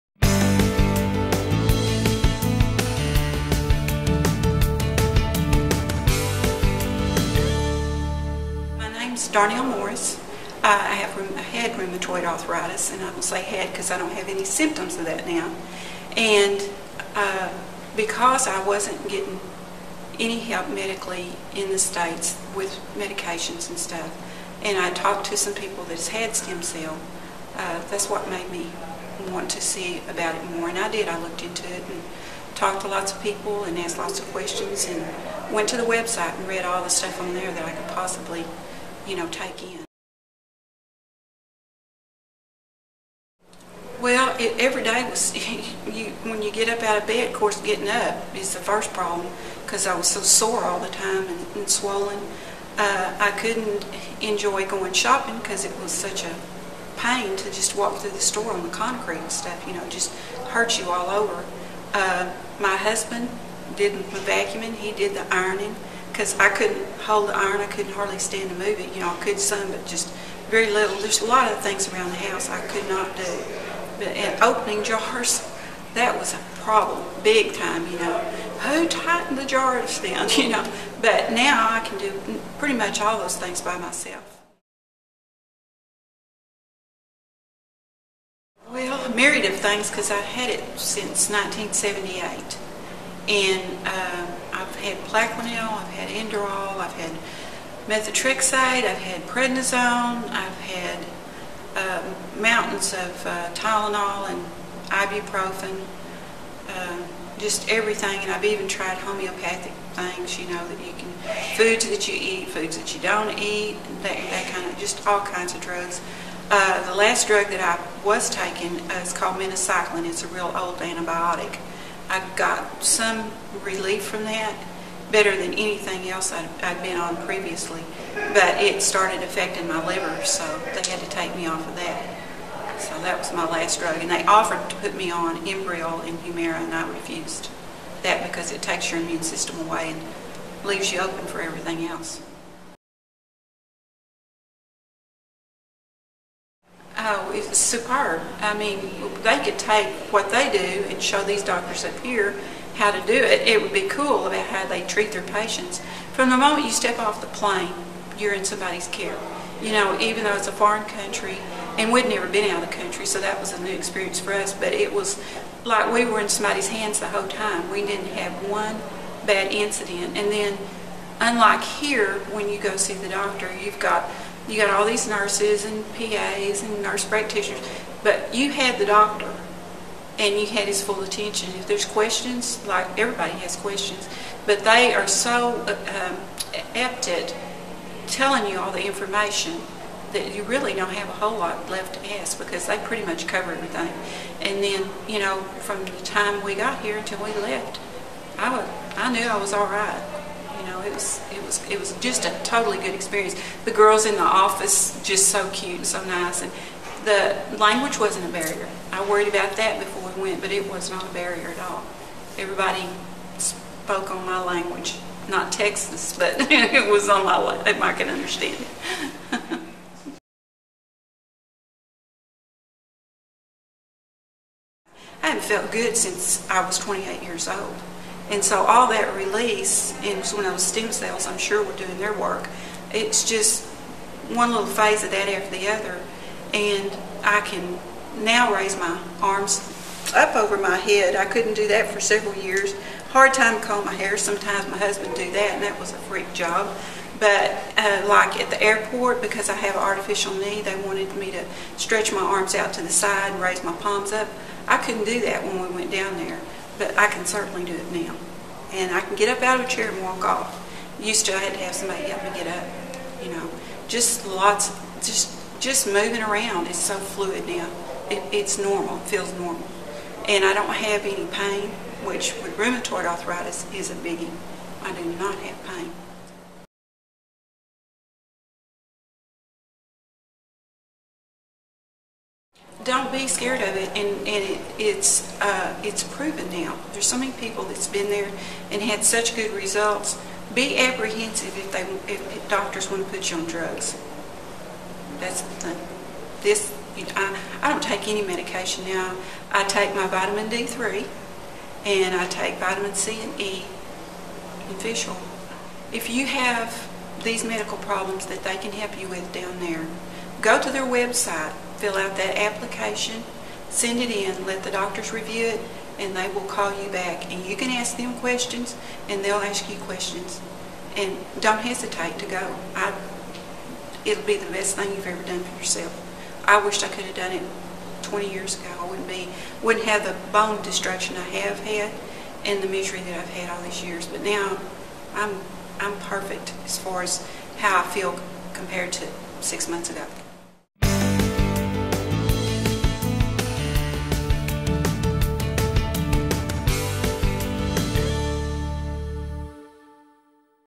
Stem Cell Treatment for Rheumatoid Arthritis - Darnell Morris
"I don't have any symptoms now." - Mrs. Morris discusses how stem cell therapy has improved her life; greatly increasing her range of motion and eliminating her pain.  Mrs. Morris suffered with Rheumatoid Arthritis for over 30 years.  She also explains how RA affected her before stem cell therapy including the laundry list of drugs shes taken over the years.